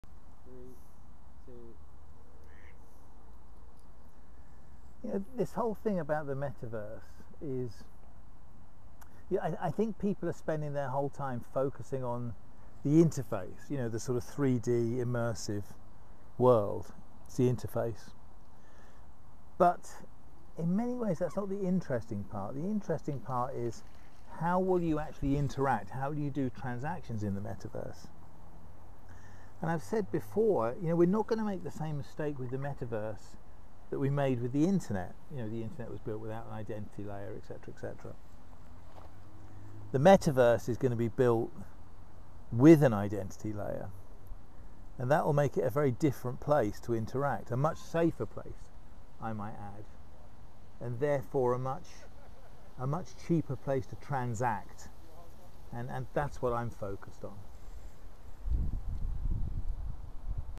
LI 074 Metaverse Mistake
It's a mistake to think that the metaverse has failed.